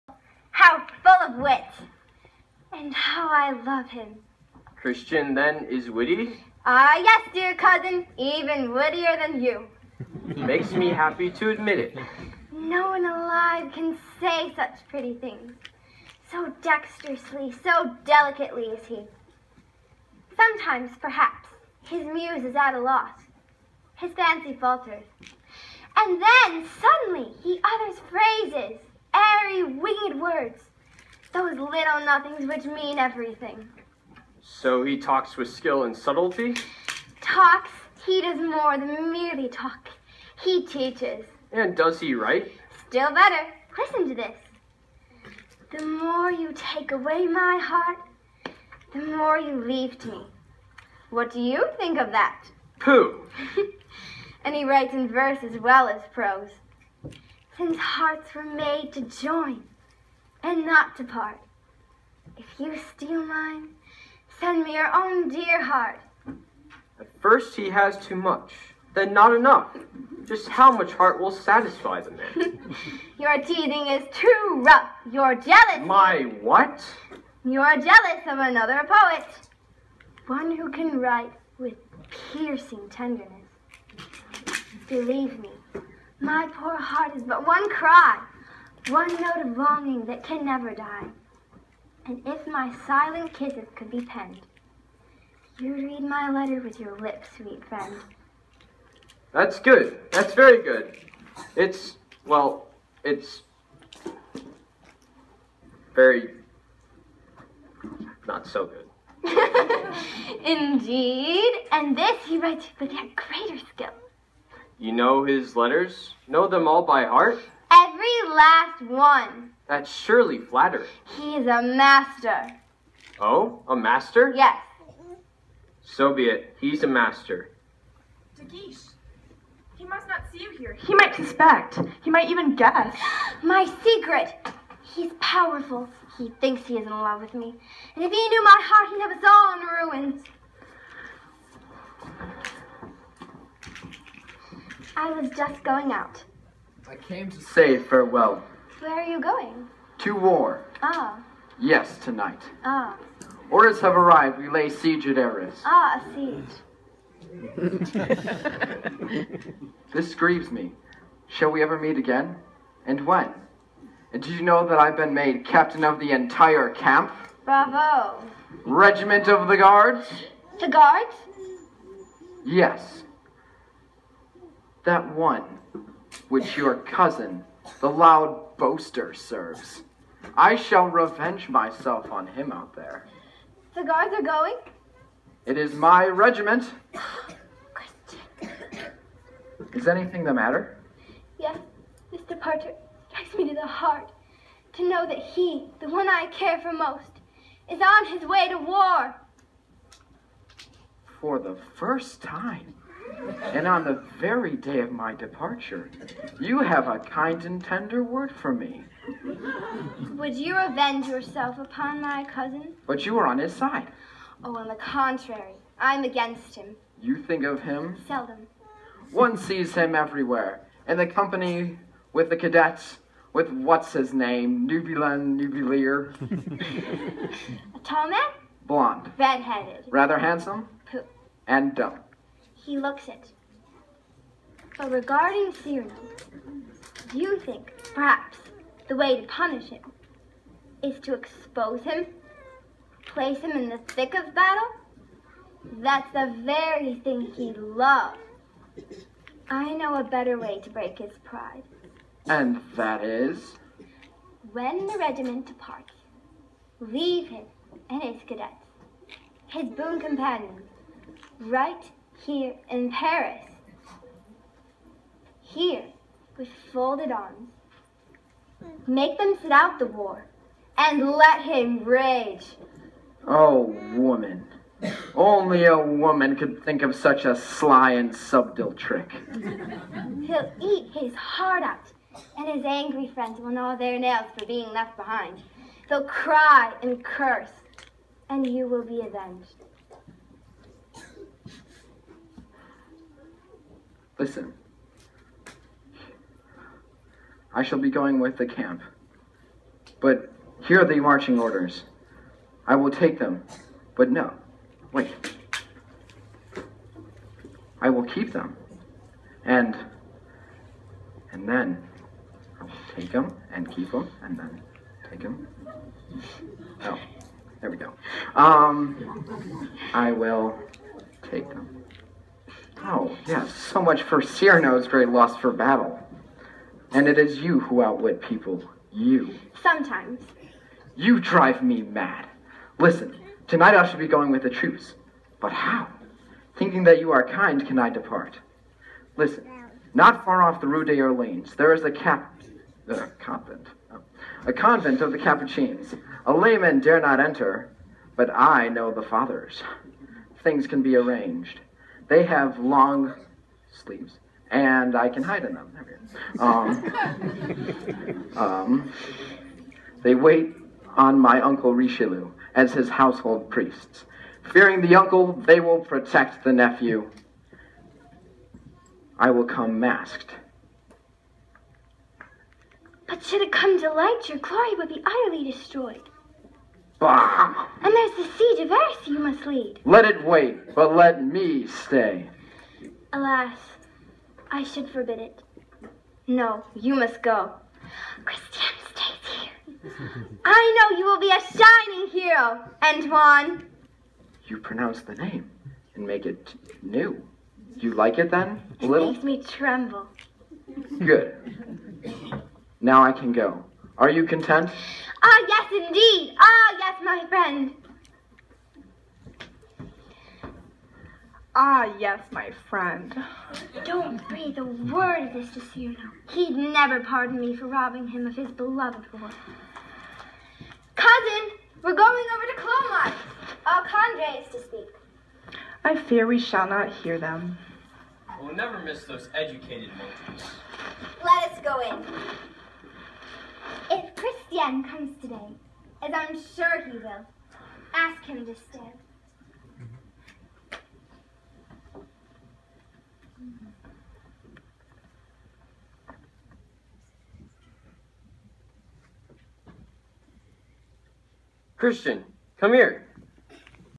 Cyrano de Bergerac play, Chesterton Academy sophomore class 01-17
Regina McLain playing Roxanne